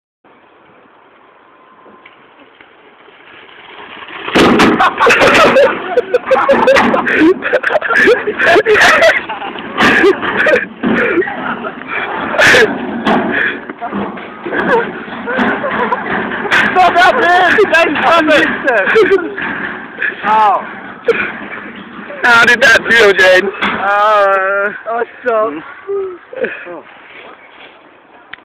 HAHA funny
my m8 runs int a bin and gets smashed